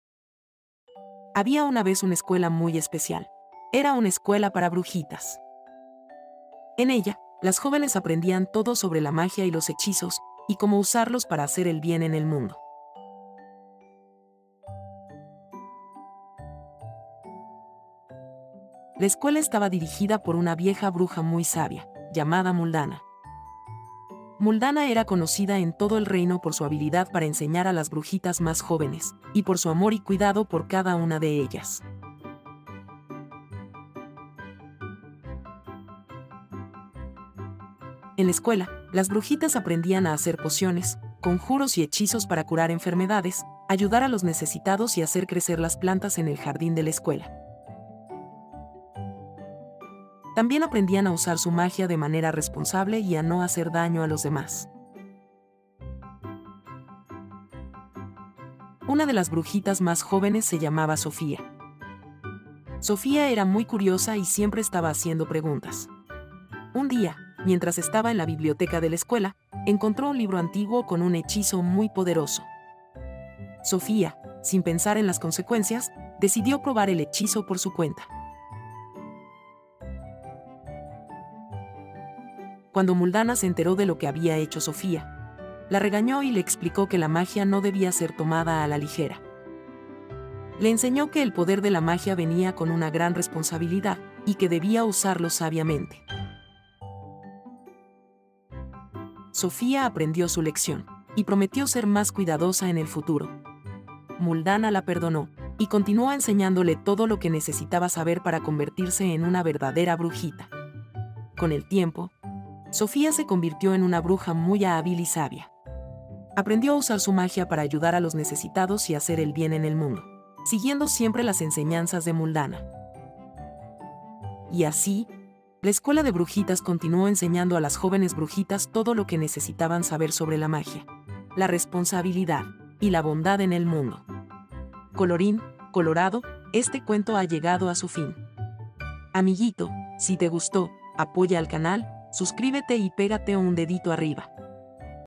📚CUENTOS CORTOS COLORIDOS- ESCUELA MAGICA
En la escuela mágica se aprendían muchas cosas maravillosas, dirigida por su sabia maestra Muldana, alli las pequeñas brujitas vivían momentos emocionantes y aprendían la importancia de la responsabilidad.

Si te gusta nuestros videos, pégate un dedito arriba y subscríbete.

🌟 Gracias amiguito!!  🤗📚

Autor: Waynes Jones